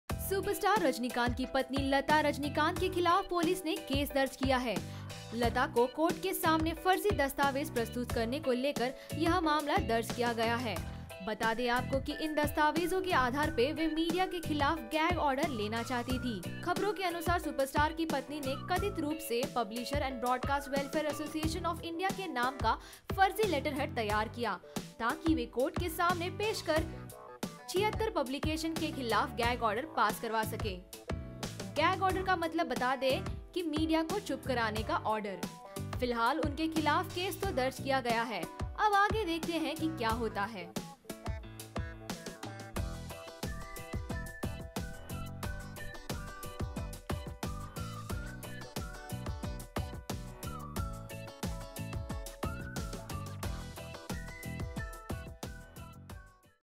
FIR filed against Rajinikanth's Wife14 Million fraud in Forgery case
The Bengaluru police have filed an FIR against Tamil superstar Rajinikanth's wife Latha in an alleged case of forgery and using fake documents.►Circle us on Google+